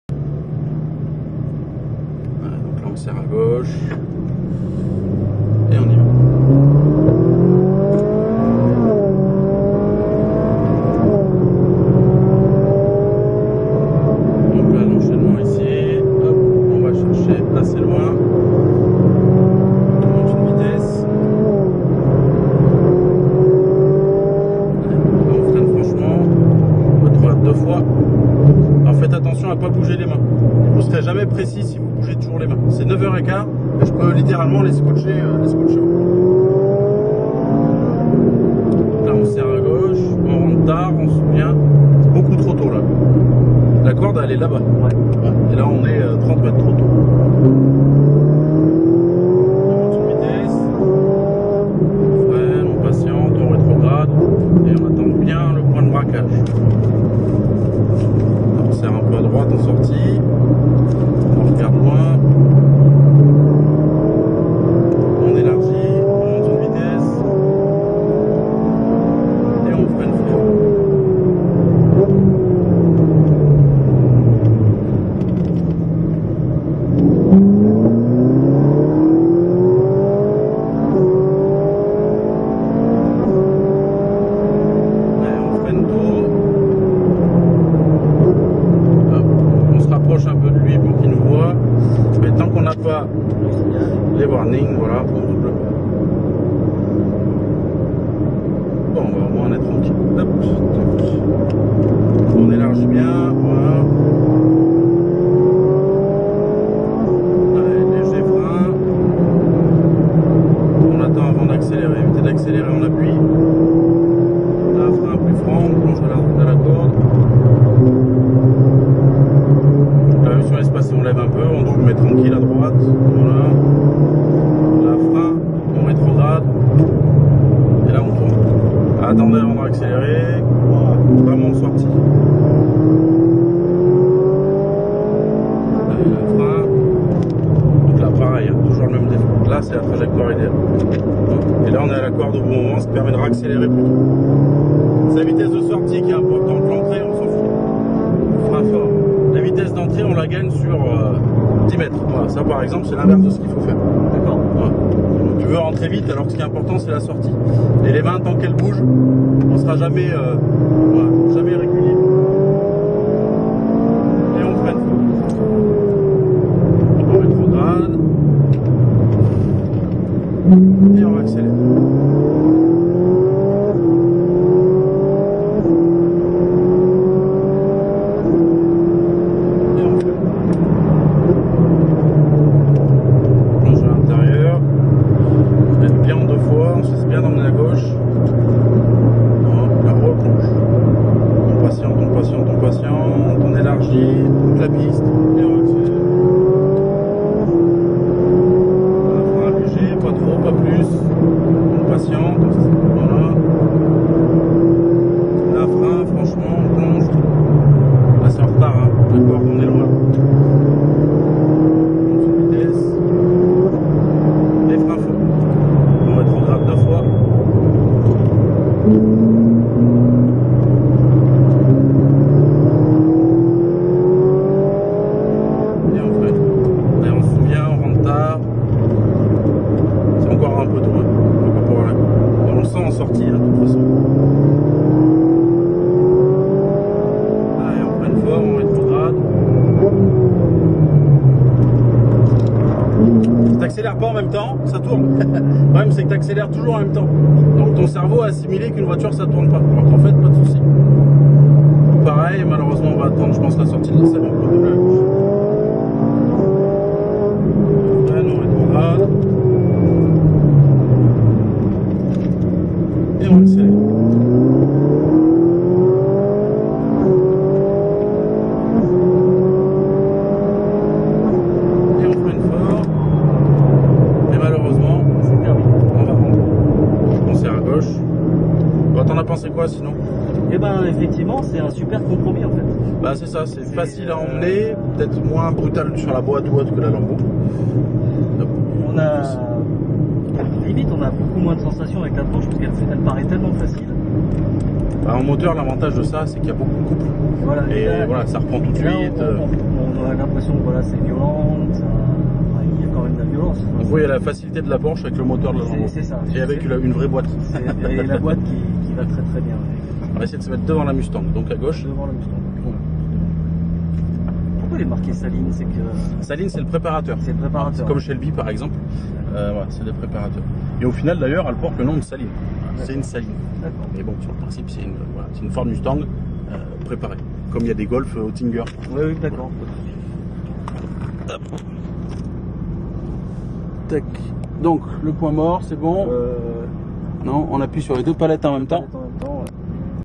F458 ITALIA circuit d'issoire
qq tours de F458 italia 570ch sur le circuit d issoire